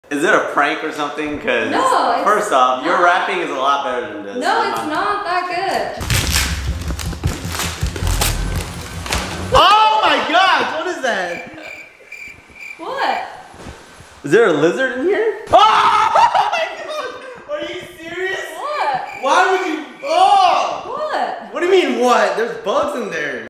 I think Stove loved his present!🎁
Today Lizzy is gifting gag gifts to all her friends. Stove opens his present which turns out to be a bunch of bugs!